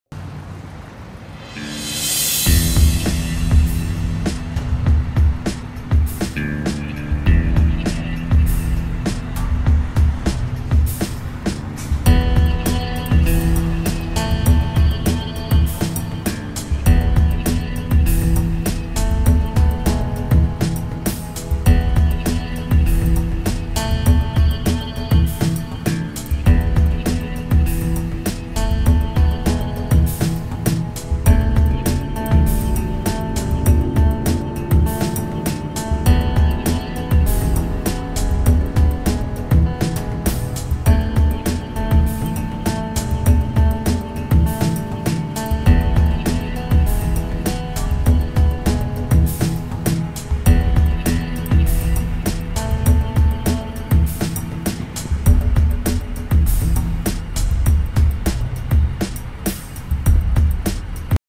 Giant Wall Cloud Eating The Clear Sky. Toledo Ohio
This monster bank of clouds moving in. Awesome. God bless.